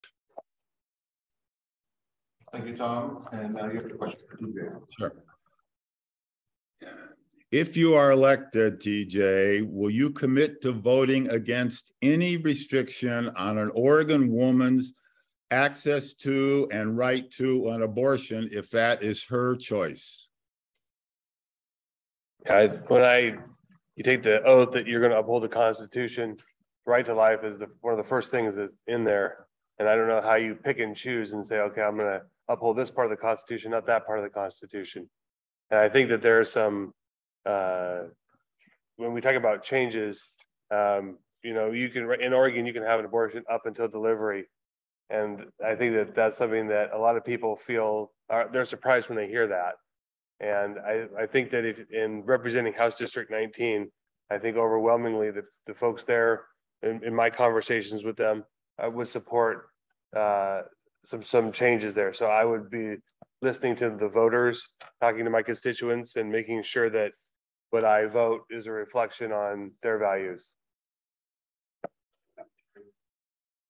TJ Sullivan says he opposes abortion rights
TJ Sullivan is running against Tom Andersen in Oregon's House District 19 race. This was Sullivan's answer to a question about abortion at a Chamber of Commerce forum.